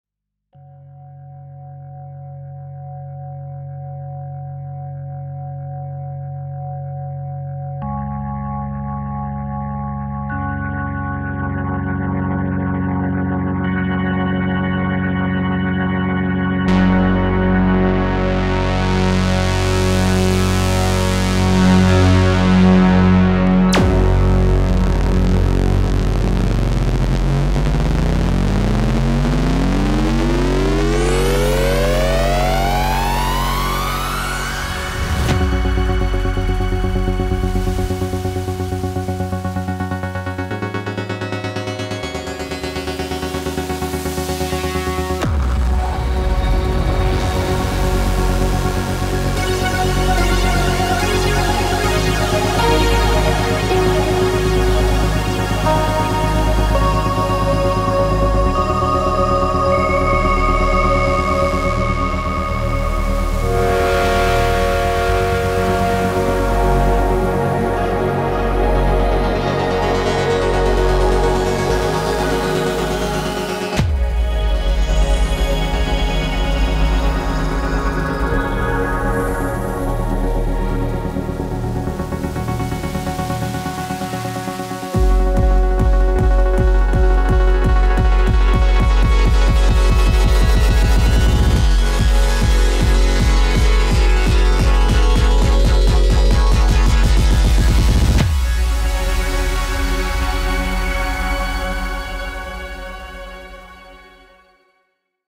V Collection 8 | Legendary Keyboards Reinvented
A quintessential anthology of the most evocative synthesizers and keyboards ever made, meticulously modeled, lovingly recreated and expertly enhanced to inspire.